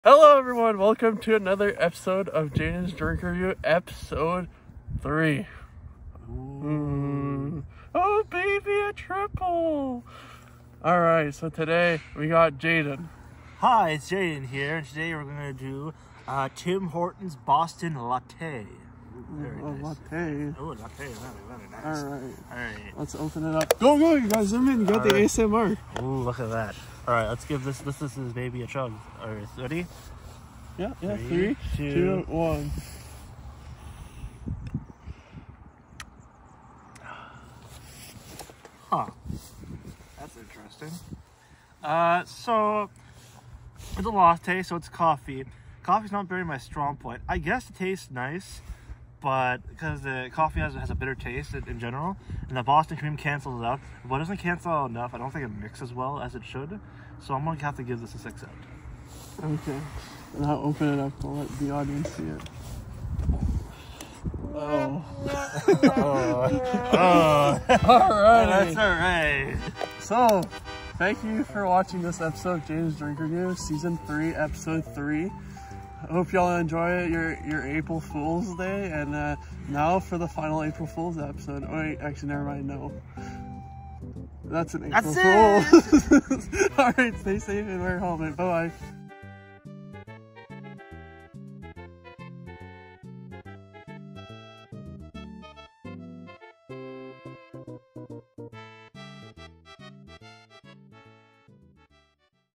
JDR: Interesting Mix! (S3E3)
Now for a Bailey's Latte review from Jaden? Enjoy!